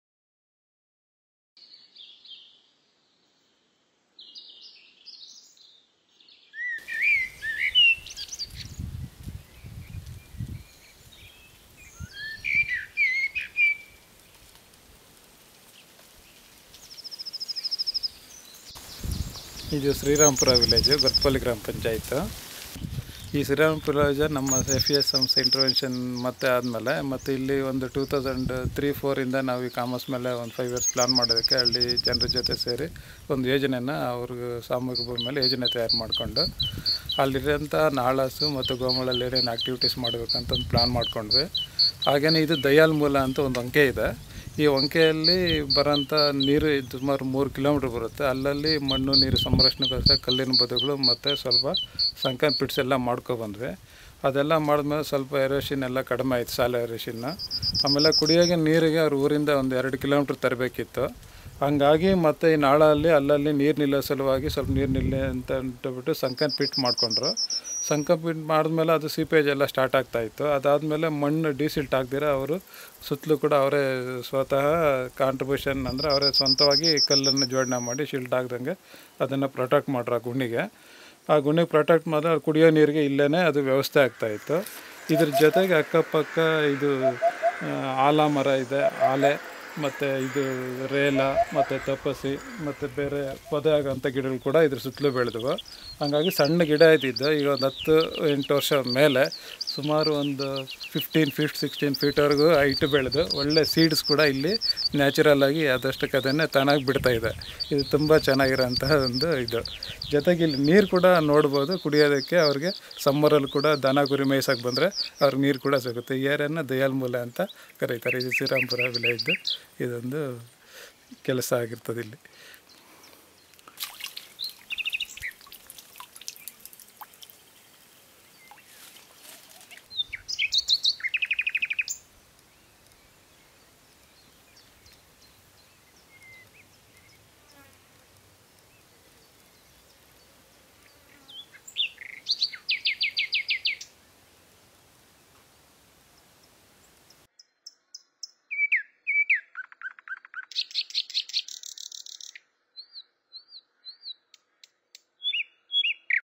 Srirampura Village, Chikballapur
Impact of conservation activities on the upper catchment of the Papagni River. The terrain is largely hilly and rocky, it supports scrub forests, grazing lands, wetlands and agricultural lands.
Sustained protection of common lands by the village institution have led to improved water levels and vegetation cover in the boulder-strewn landscape of Srirampura.